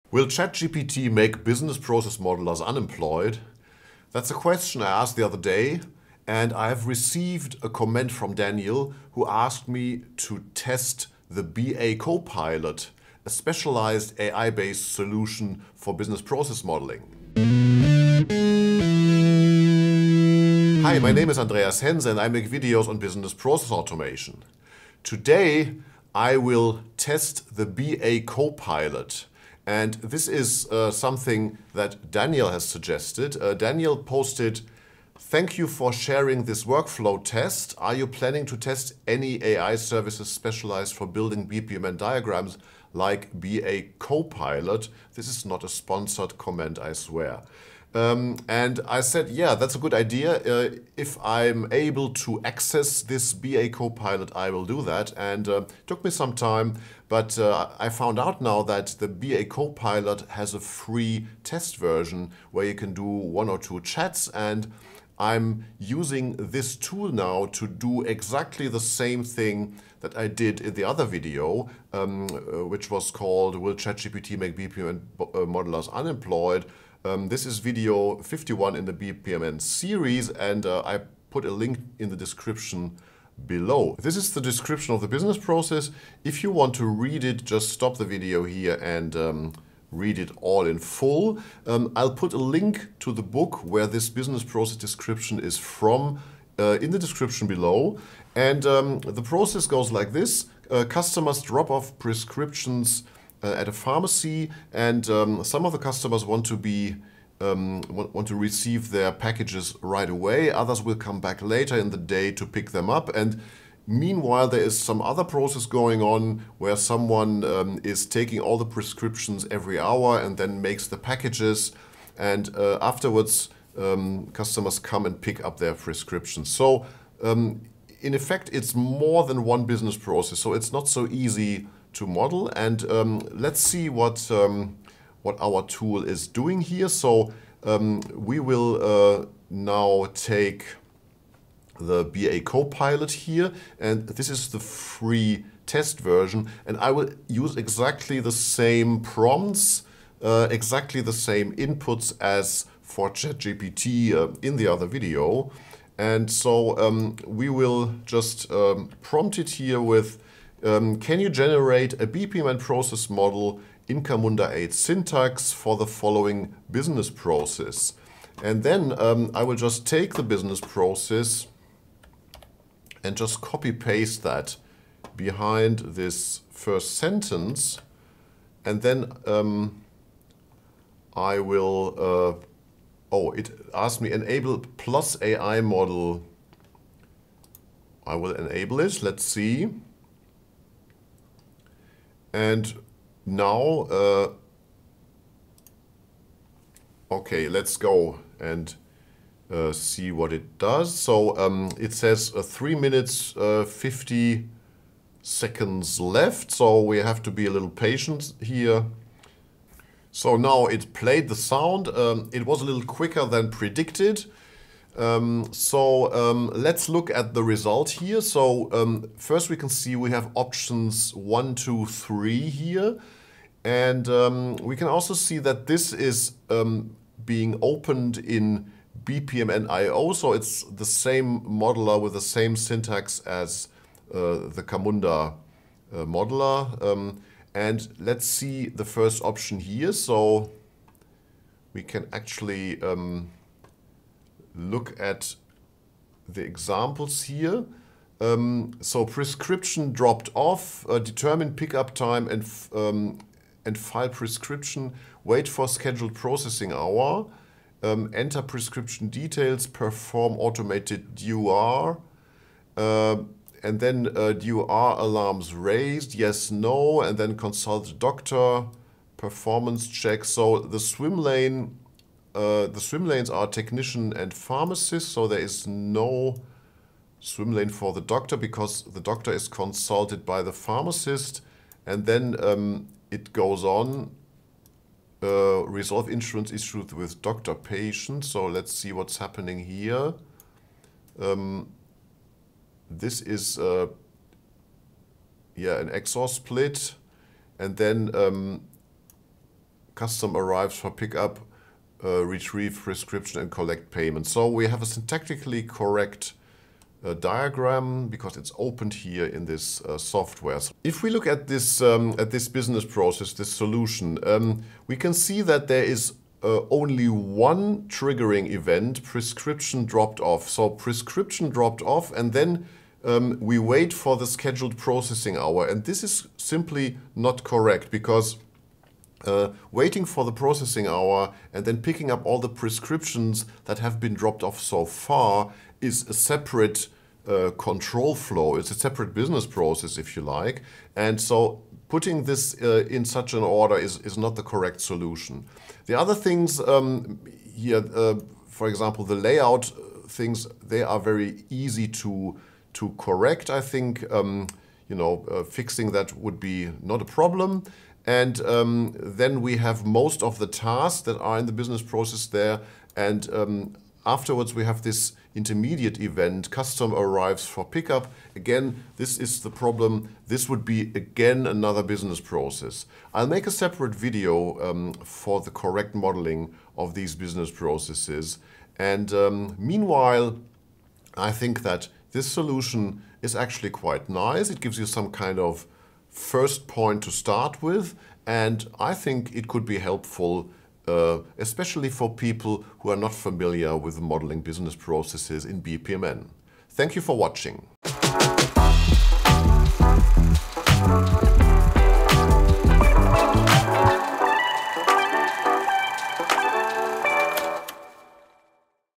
BA Copilot AI for BPMN Modeling – Can It Replace Traditional Methods?
How well does BA Copilot’s AI perform in business process modeling compared to chatGPT? The video explores whether the AI tool BA Copilot can take over the role of creating business process models. It demonstrates how the tool works by testing its capabilities in modeling workflows. The discussion highlights both the potential and limitations of using AI in business process automation.

This is video #56 of the BPMNSeries.

00:00 Introduction
01:23 Entering the Pharmacy Process
03:29 Watching the Solution of BA Copilot